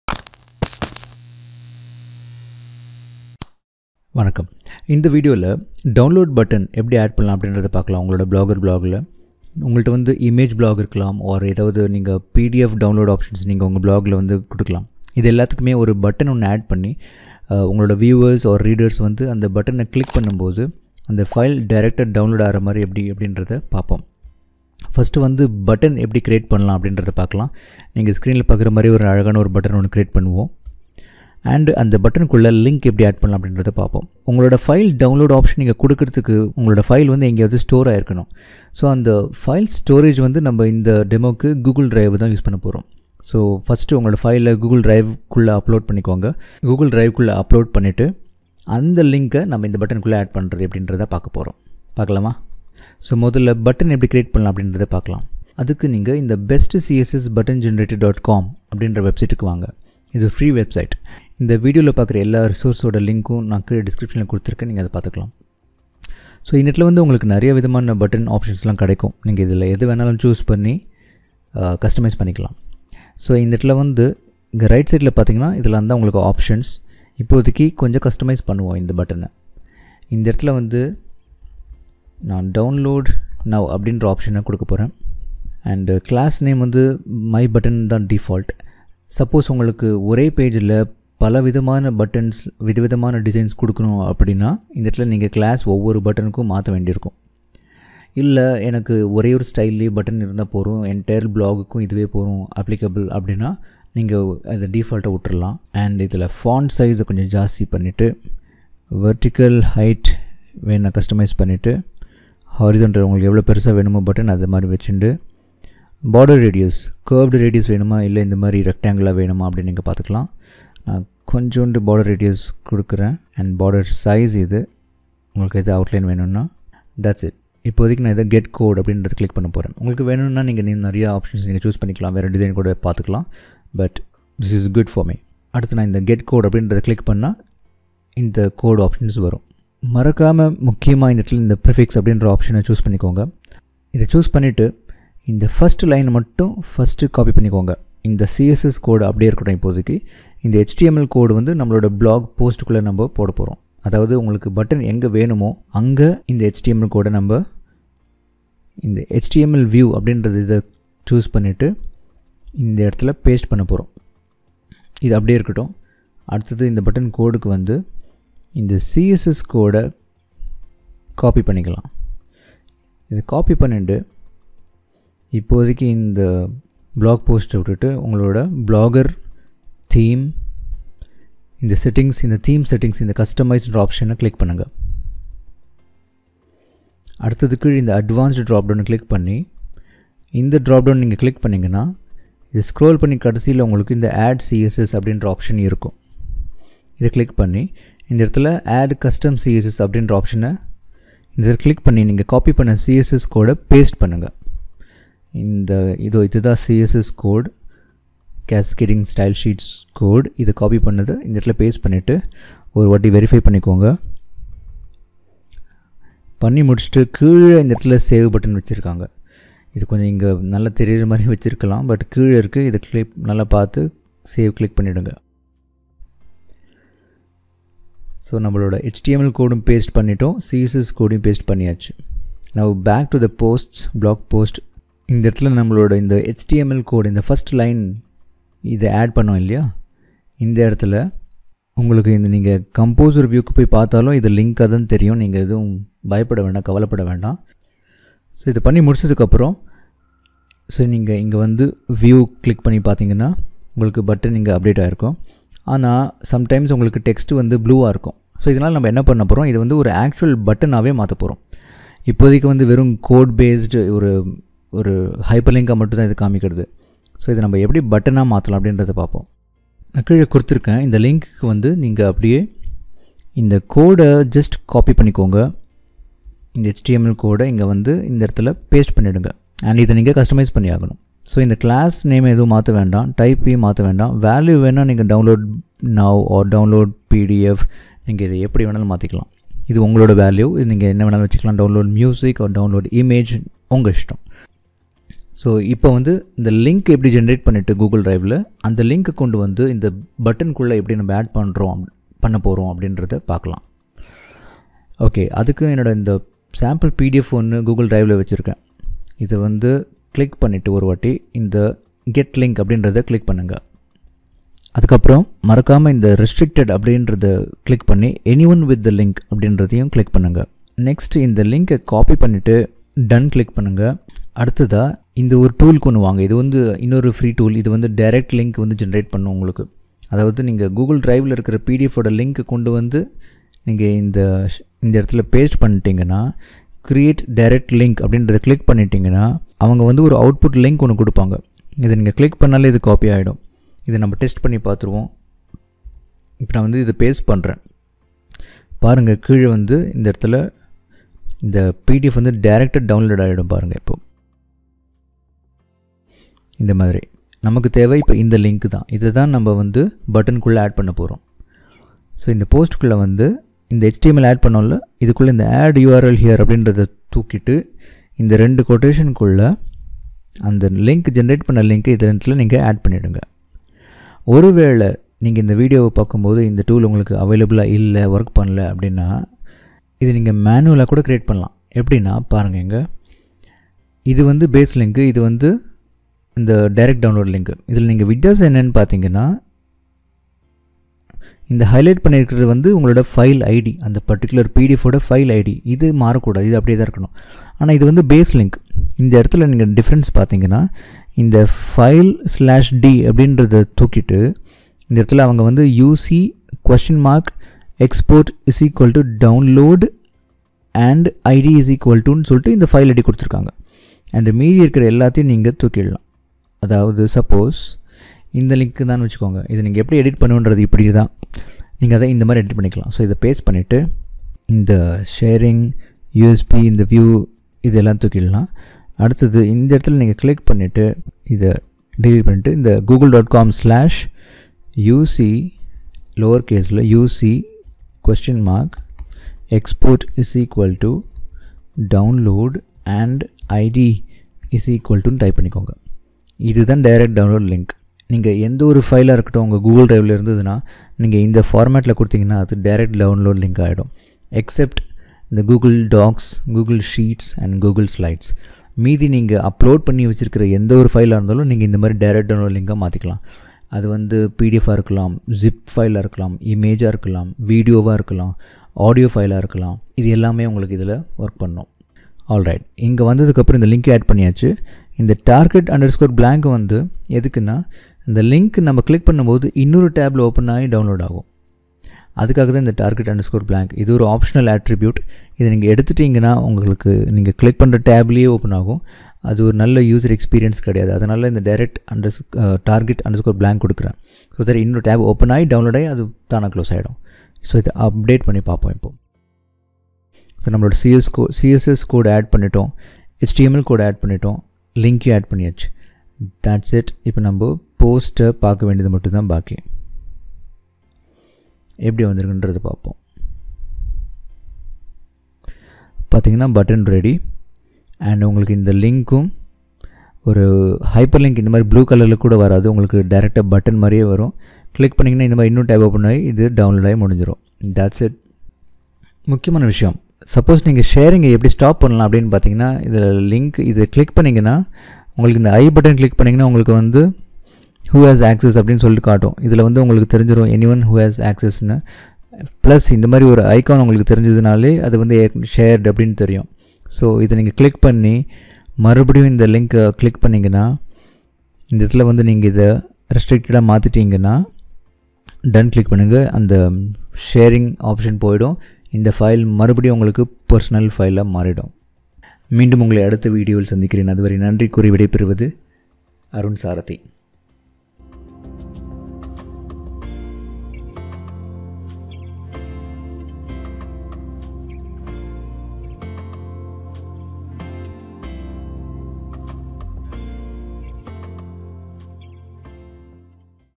How to Add Download Button in Blogger in Tamil
😍 In this video, we'll see how to add download button in Blogger, that can added to blog posts, pages, sidebar as a gadget.

### KDP, eBook writing, publishing in Indian languages,
### eBook creation and formatting process,
### WordPress and Blogger tutorials,
### Website development related blog posts.